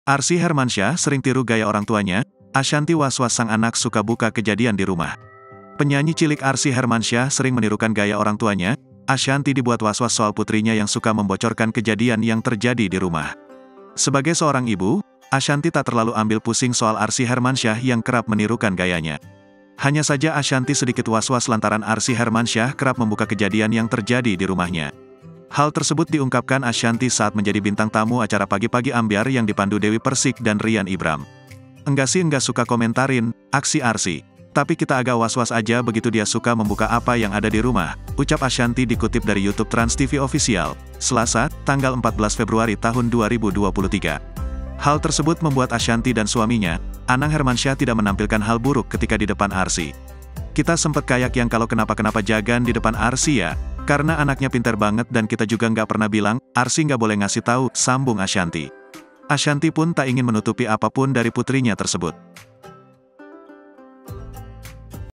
ARSY SERING TIRU GAYA ORANG TUANYA, ASHANTY WAS-WAS: SUKA BUKA KEJADIAN DI RUMAH
TRIBUNLAMPUNG.CO.ID - Arsy Hermansyah sering tiru gaya orang tuanya, Ashanty Was-was: suka buka kejadian di rumah.